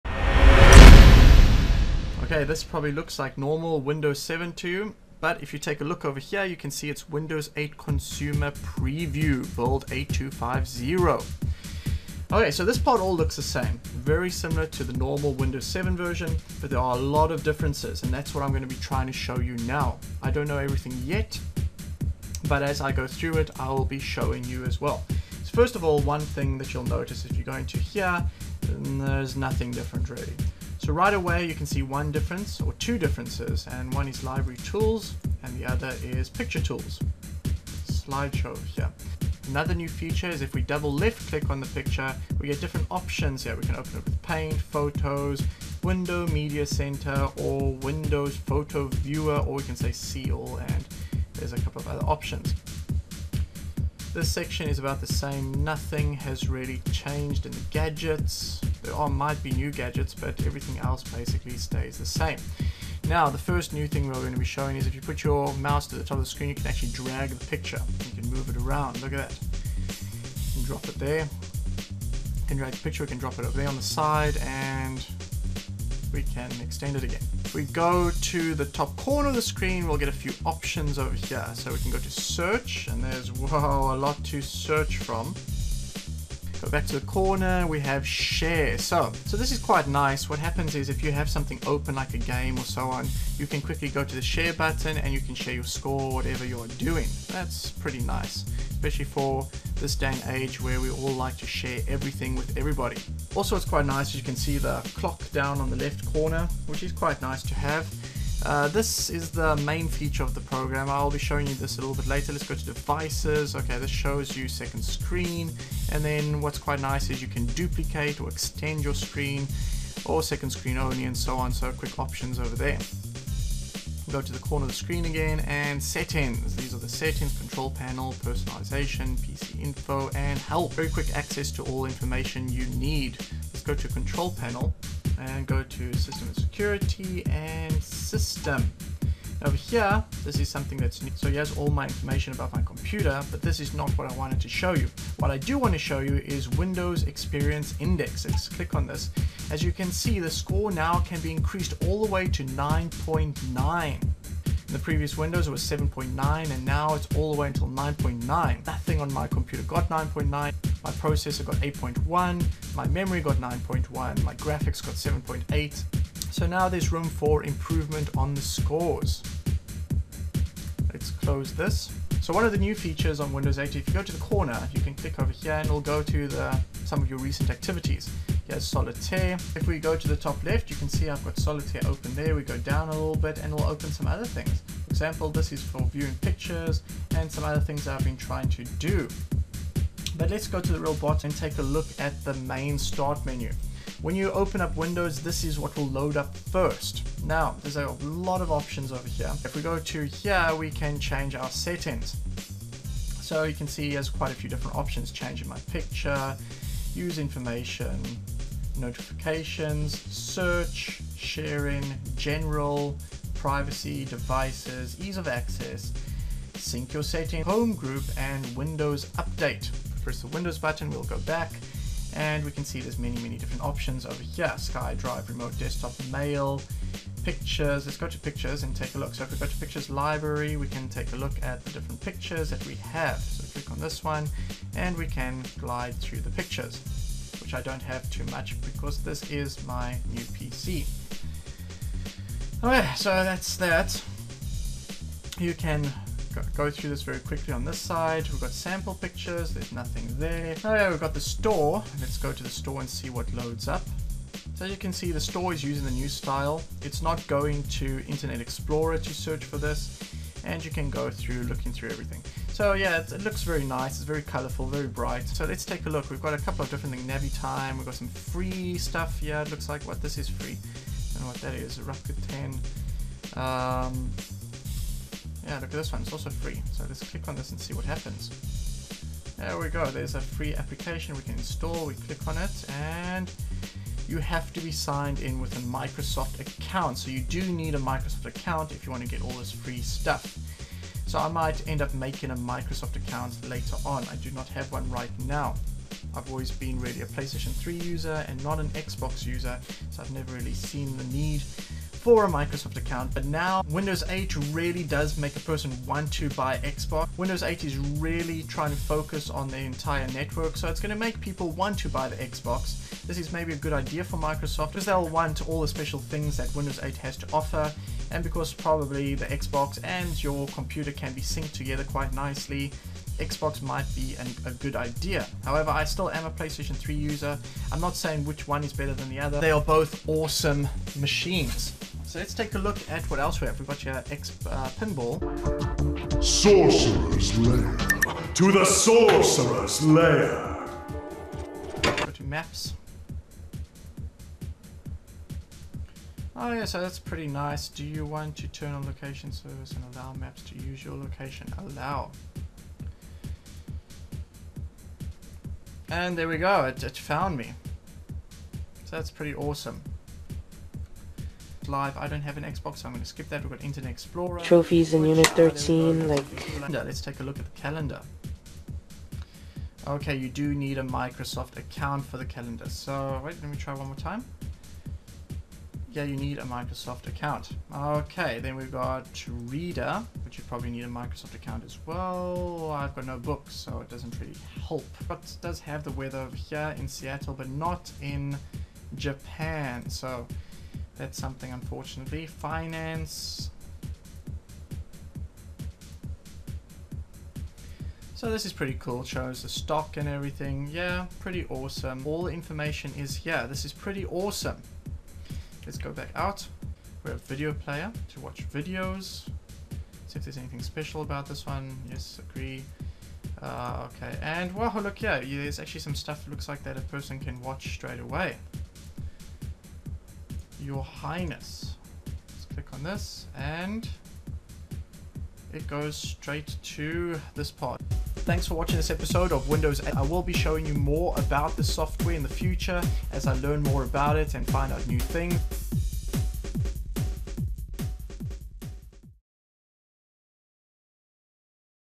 Windows 8 - What it can do (A quick look at everything)
Today we take a look at the all new Windows 8 operating system. This is to give anyone who is interested in windows 8 a quick glance at all of its features and what is new and for all the information you are probably looking for.

Windows 8 is scheduled for release in October, 2012.